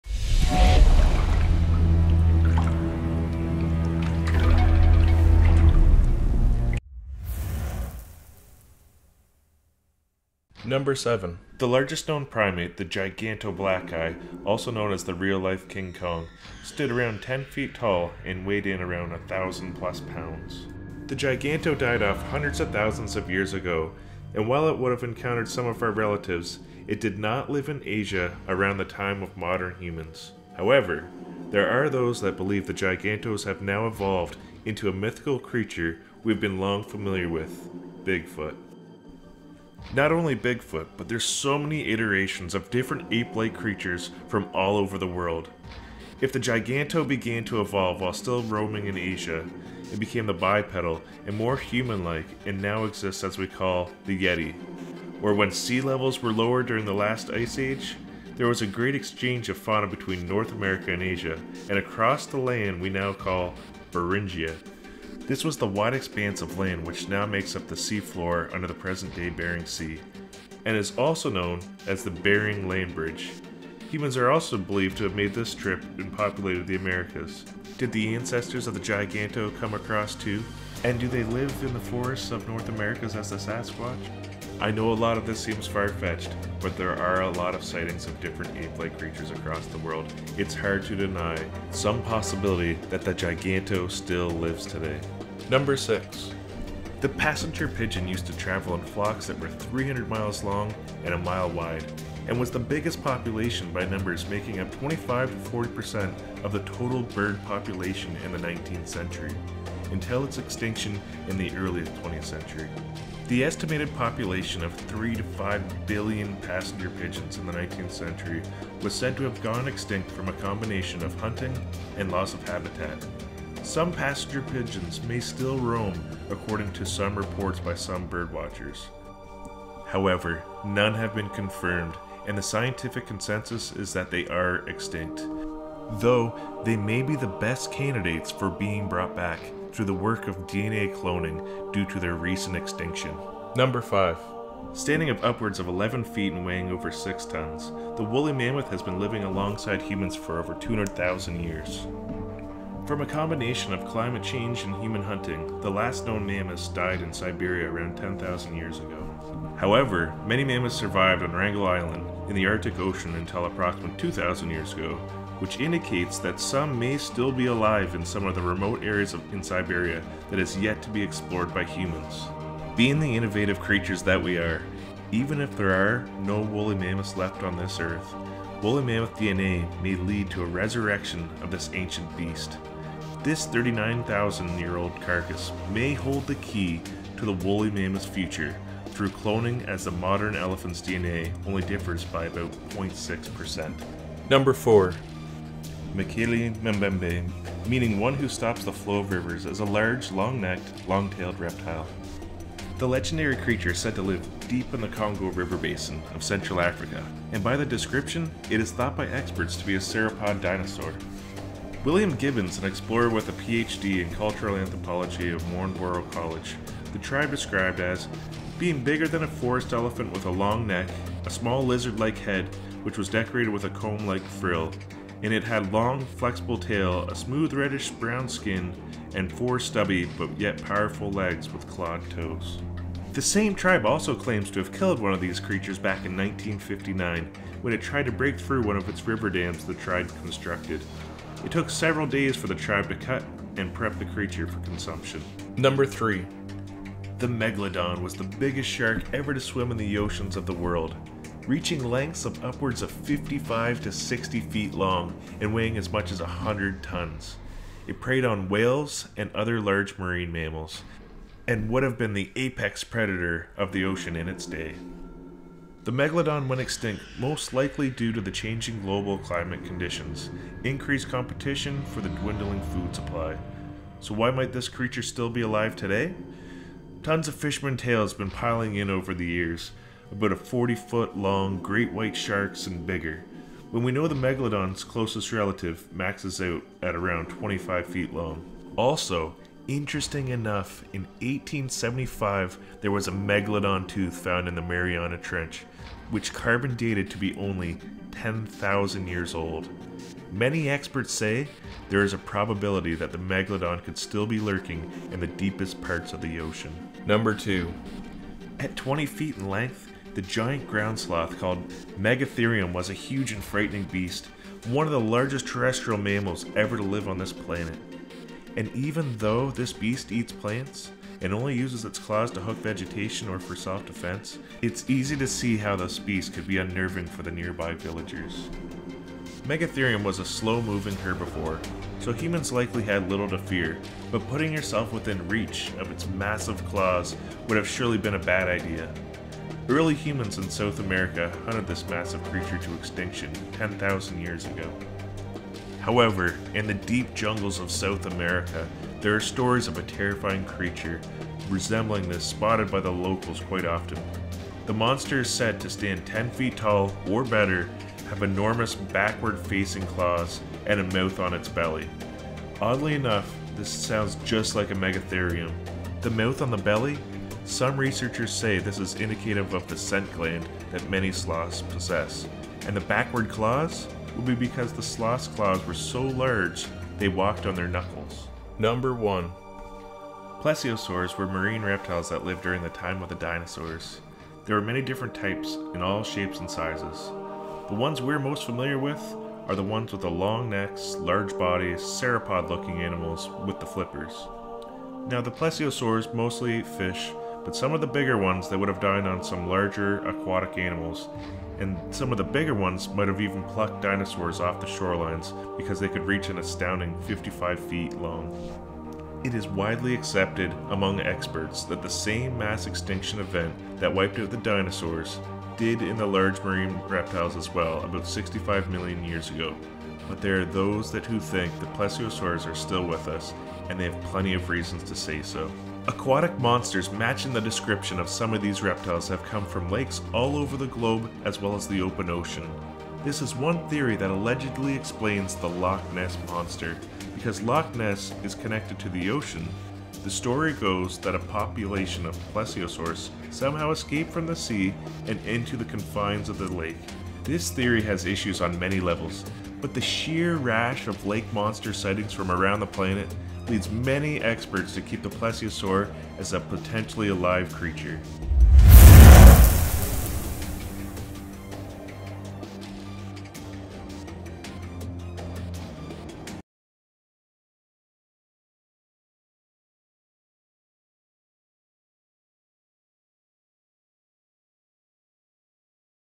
7 Extinct Animals That May Still Be Alive
7 Extinct Animals That May Still Roam The Earth Today in some remote parts of the earth.  Even if no live species are found for some of the animals, they can be resurrected with modern day science.